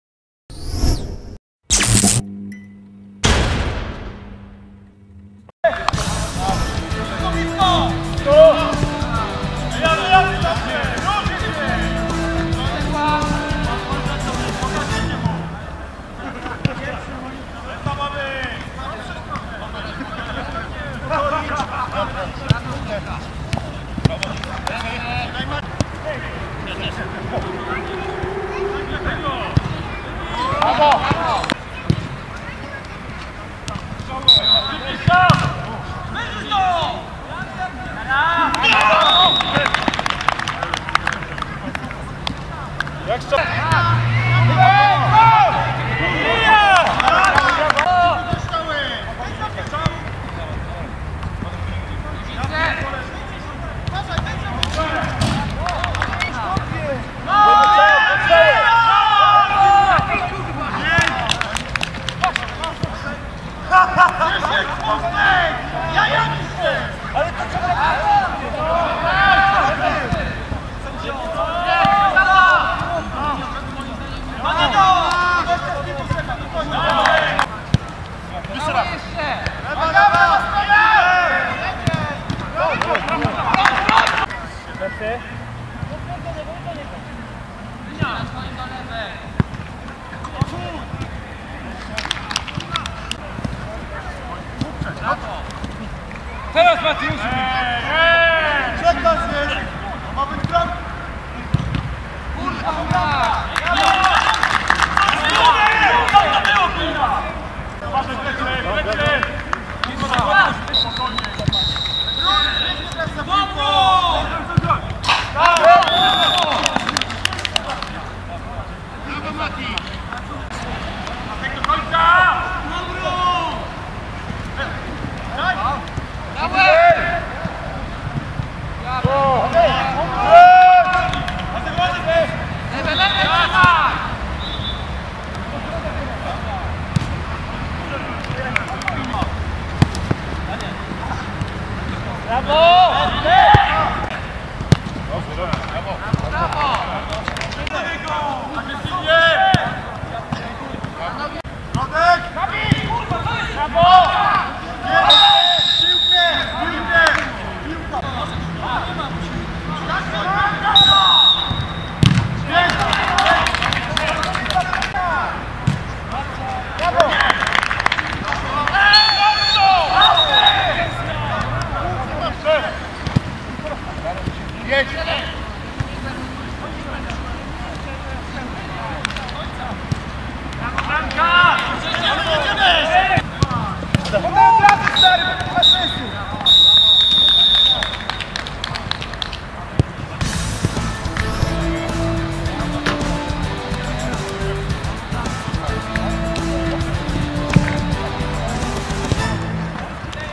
5. tydzień: PGD Grupa - Piraci z Karaibów (FLS Jesień 2011)
Skrót meczu PGD Grupa - Piraci z Karaibów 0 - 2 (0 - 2)